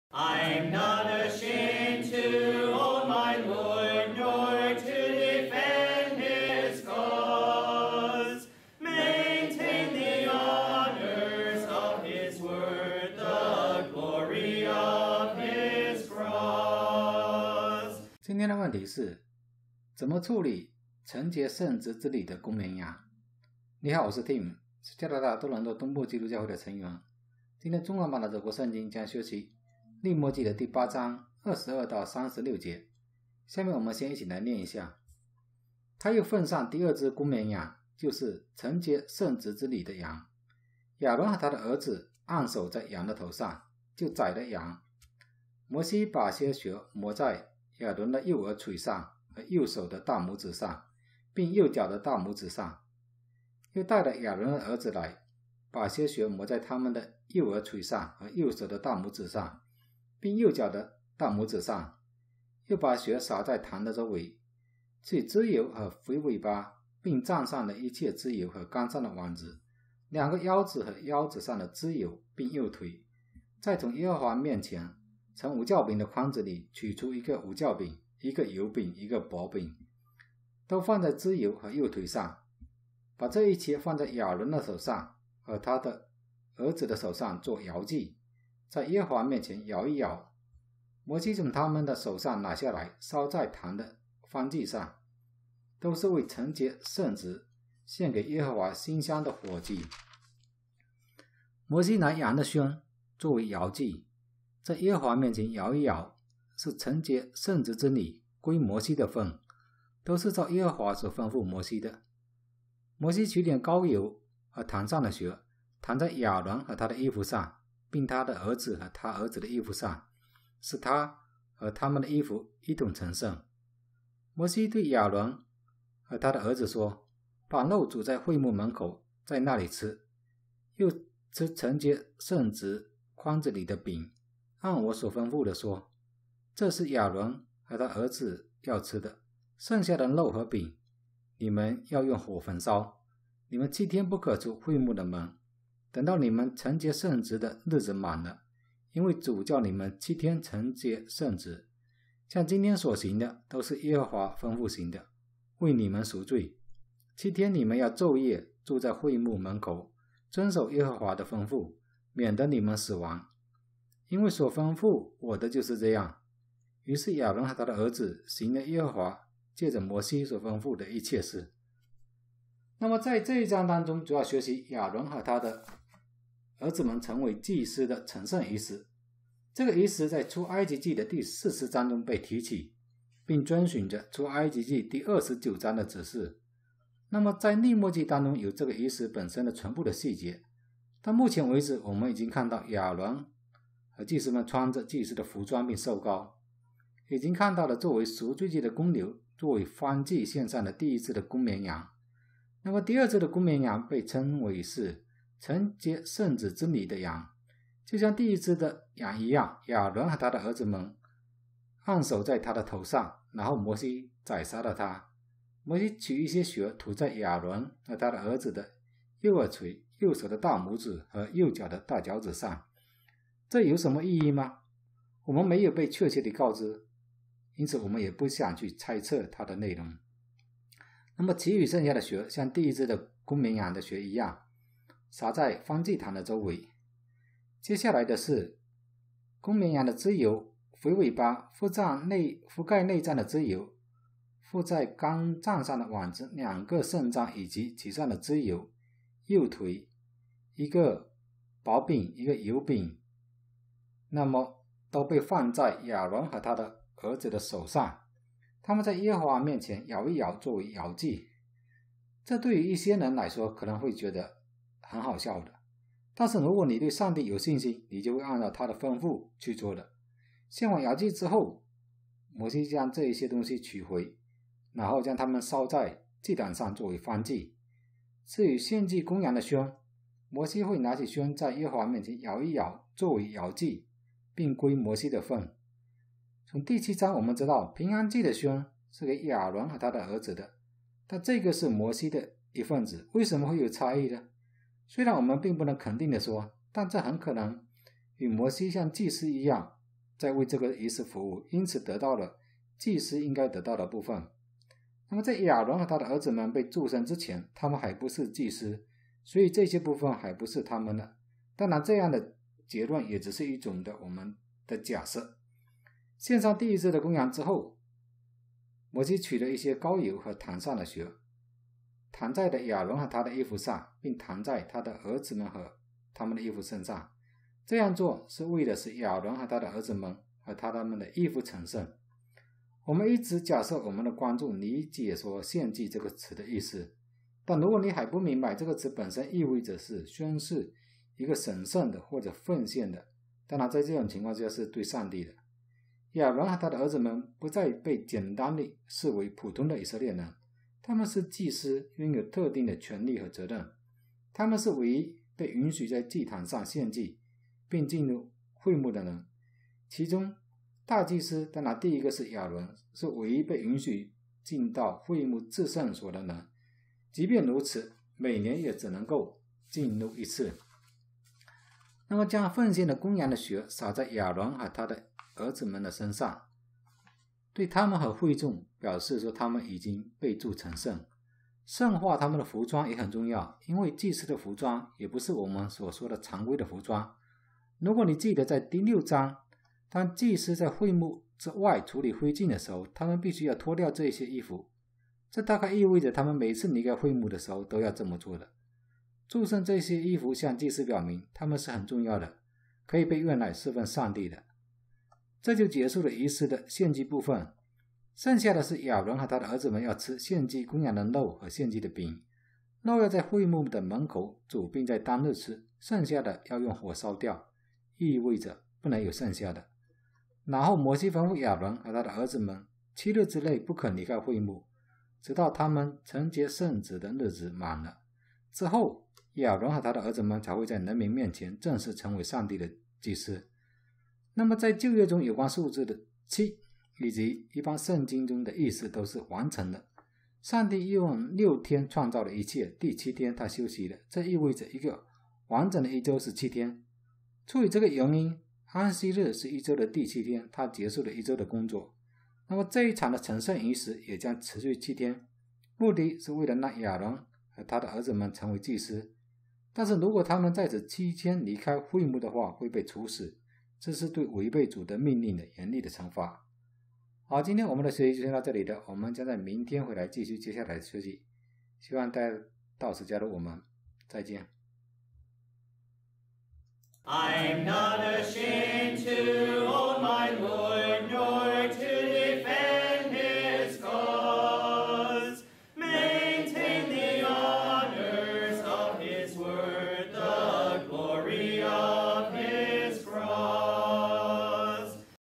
怎么处理承接圣职之礼的公绵羊？[利未记8:22-36][第18课]
怎么处理承接圣职之礼的公绵羊？这是我们将在利未记系列的第18课中回答的问题。

该播客由加拿大多伦多的东方基督教会组成。我们将在周一，周二，周三和周四发布新的播客。所有剧集都可以在我们的网站（